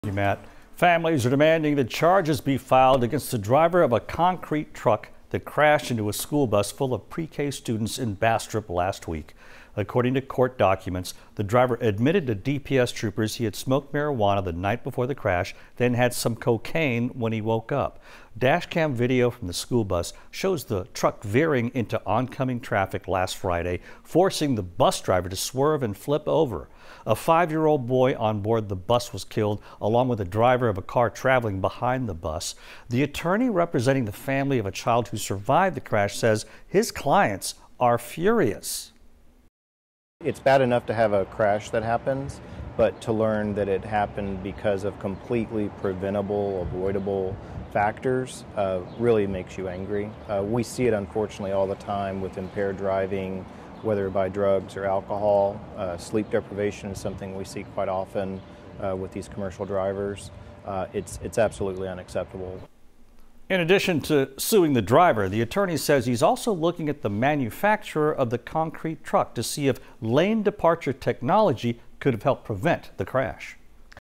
Truck driver allegedly admitted to drug use before deadly school bus crash in Bastrop County
The driver also refused to provide a blood sample for analysis to authorities.